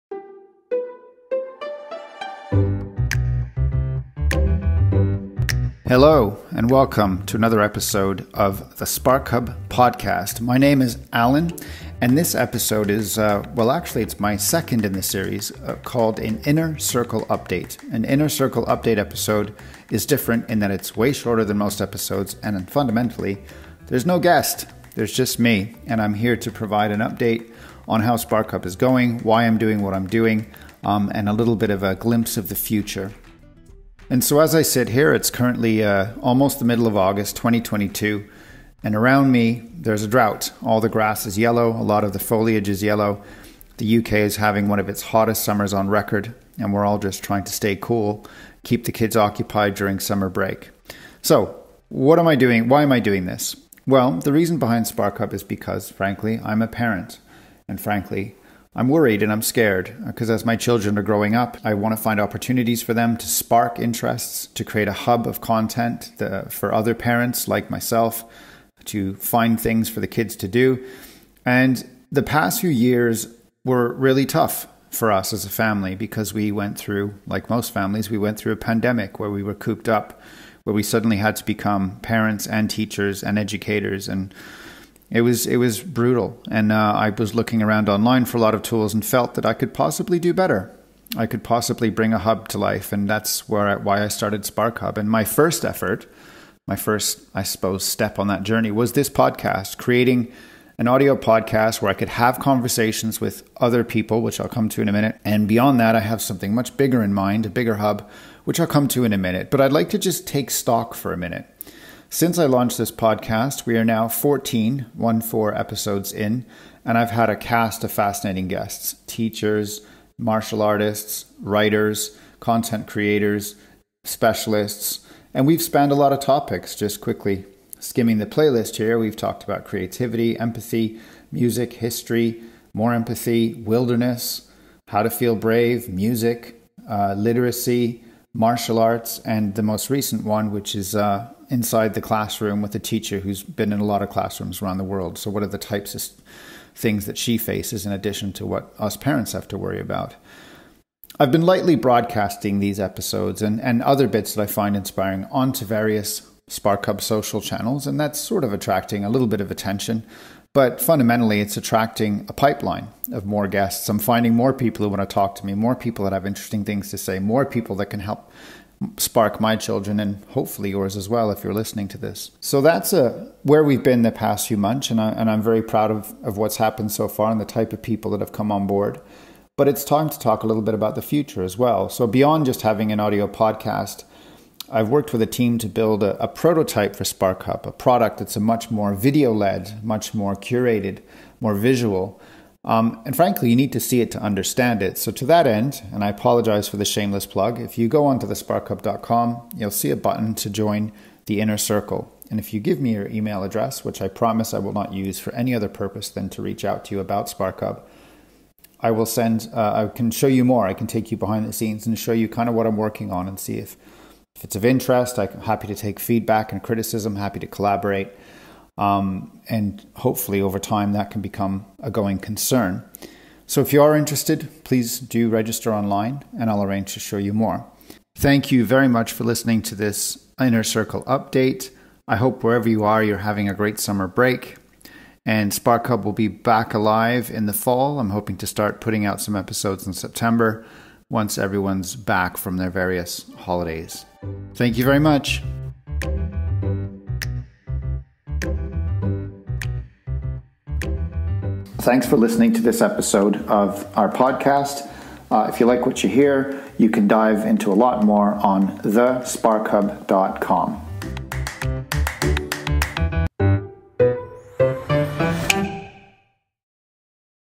Ep#15 - Inner Circle update Summer 2022 (BUSINESS)
A personal “inner circle” update from Allan Wills, cofounder of SparkHub. A review of why we’re doing SparkHub, how the podcast has gone, and what the plans for the future are - starting this fall.

Liked that? Dive into more